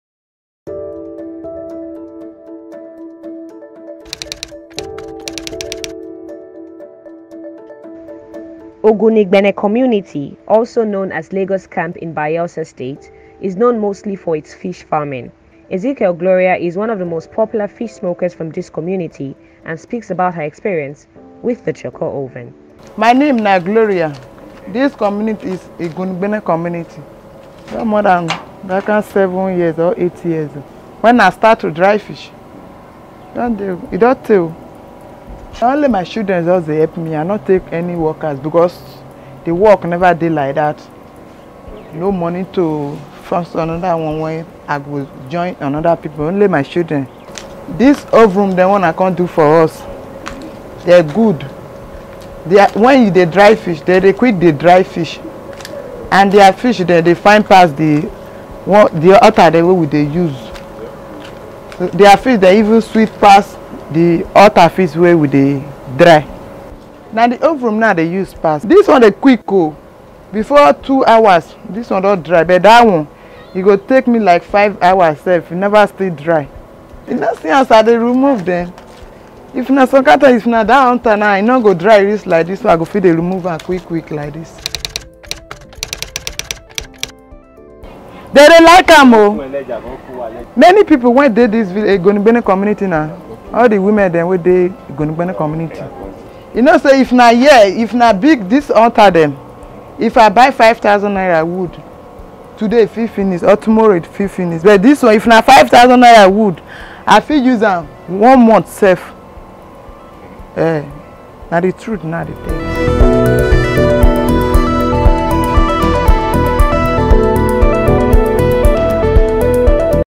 Chorkor Oven: Changing the Lives of Women in Fish Smoking in the Niger Delta; Gloria Ezekiel’s Story
Gloria Ezekiel is a fish smoker in a riverine community, Ogunigbene, in Bayelsa State who has been processing fish for about 8 years using the traditional smoking technology. However, since her adoption of the Chorkor Oven, an appropriate technology for fish smoking, she has made a tremendous decrease in production costs by using less firewood to smoke the fishes and she is able to smoke even more volumes of fishes using the same number of woods in a shorter time period compared to the traditional smoking technology. Here is her testimonial story.